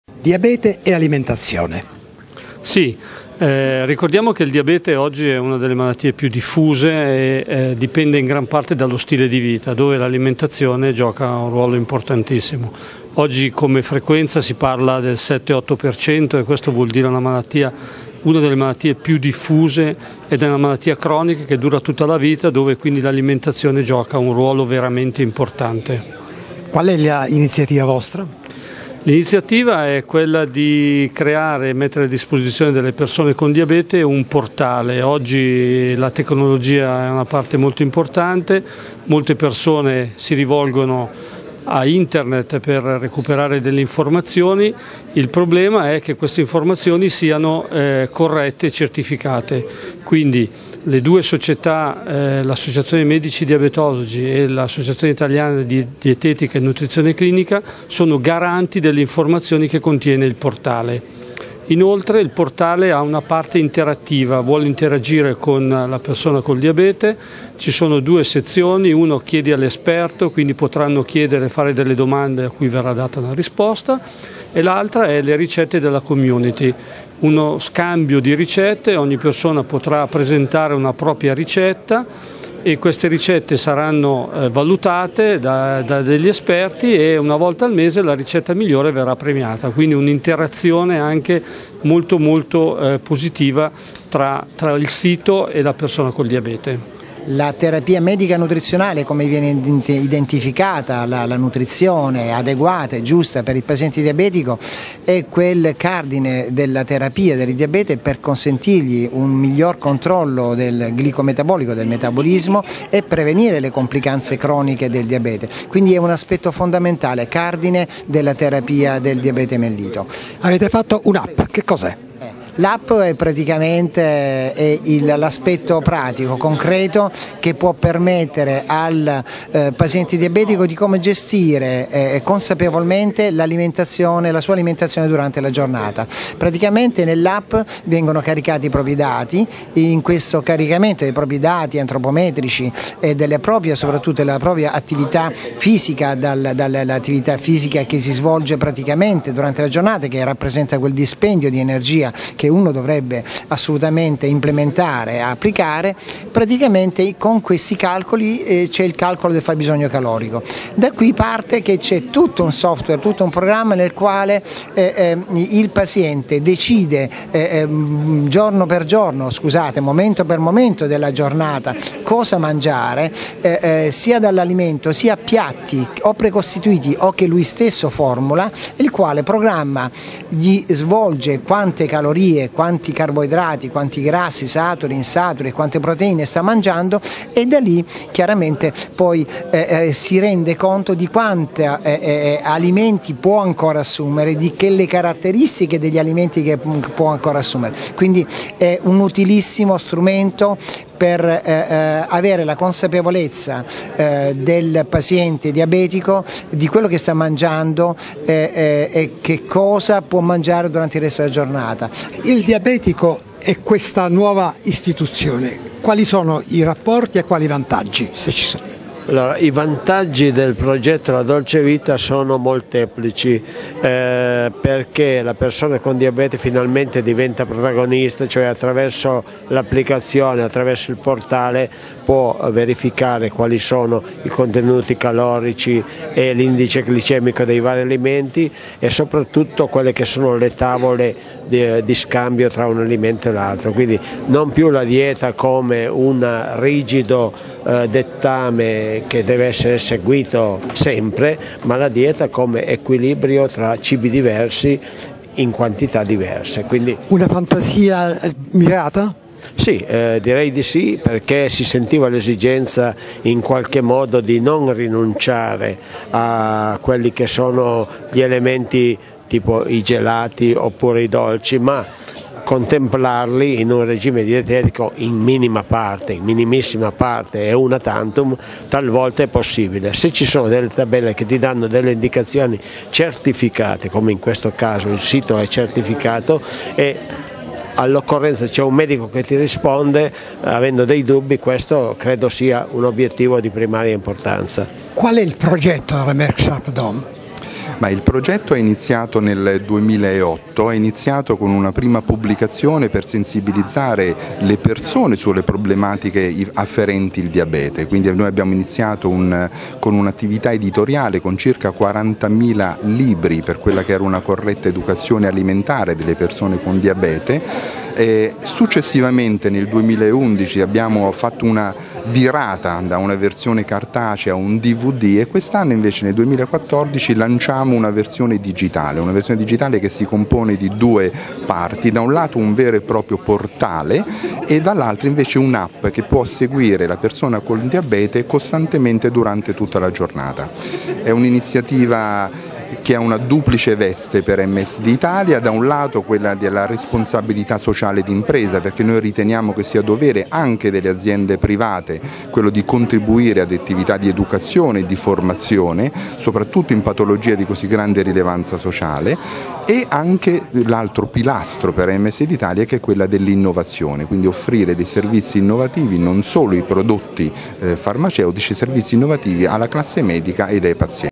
Diabete, La Dolce Vita 2.0
MSD Italia, ADI, AMD e FAND rinnovano il proprio impegno educazionale a sostegno di un corretto stile alimentare in caso di diabete e lanciano La Dolce Vita 2.0: con un’applicazione che fornisce uno strumento personalizzato ad ogni utente e un portale con ricette econsigli utili di nutrizionisti e diabetologi.

Giuseppe Marelli, Responsabile della Diabetologia dell'Azienda Ospedaliera di Desio e Vimercate;

Antonio Caretto, Segretario Generale dell'ADI, Associazione Italiana di dietetica e nutrizione;

Egidio Archero, Presidente di FAND;

Goffredo Freddi responsabile comunicazione MSD Italia.